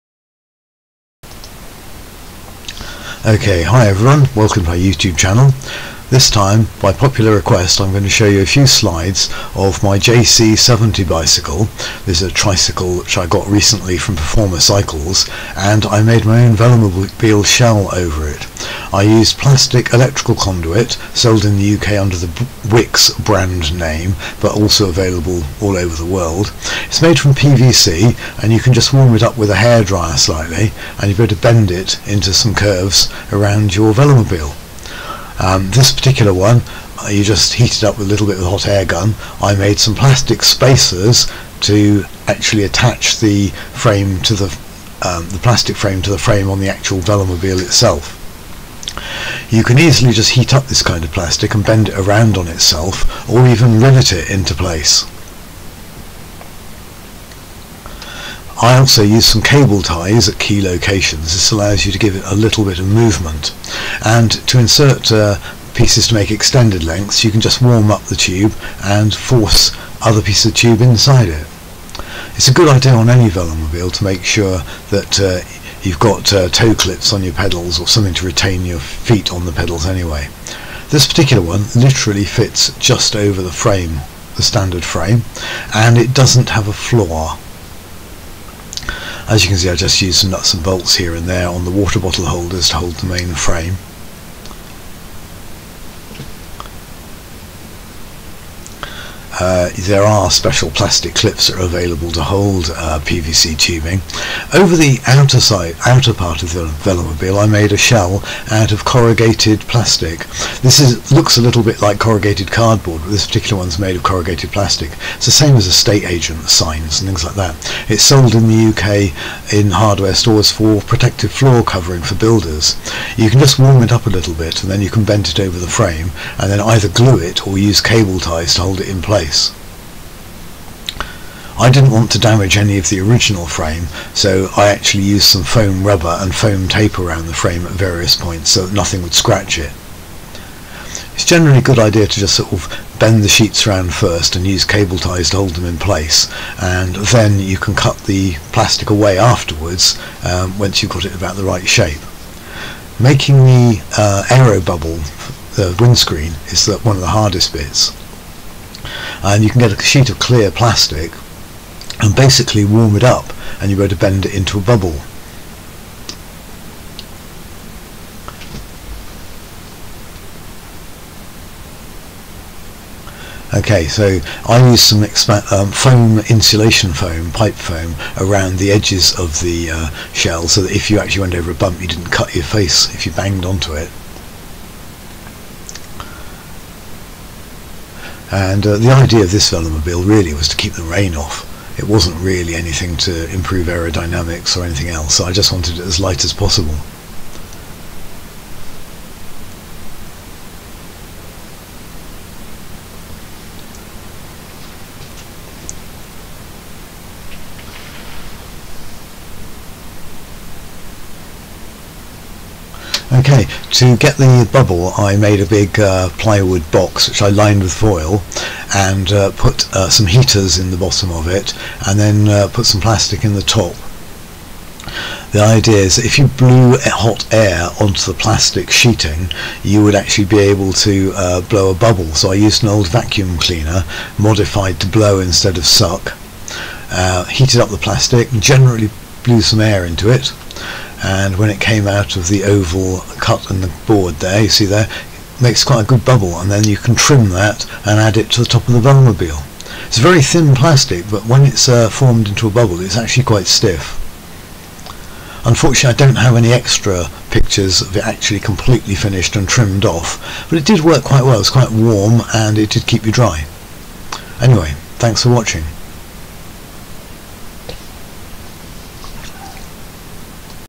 Photo montage of my DIY velomobile based on a Performer JC70 recumbent trike
A simple velomobile based on a recumbent trike. Frame made of PVC electrical conduit and cable ties. The shell is made from corrugated plastic. Cheap, effective, warm and dry. So good, I decided to splurge the cash to buy a commercial velomobile instead.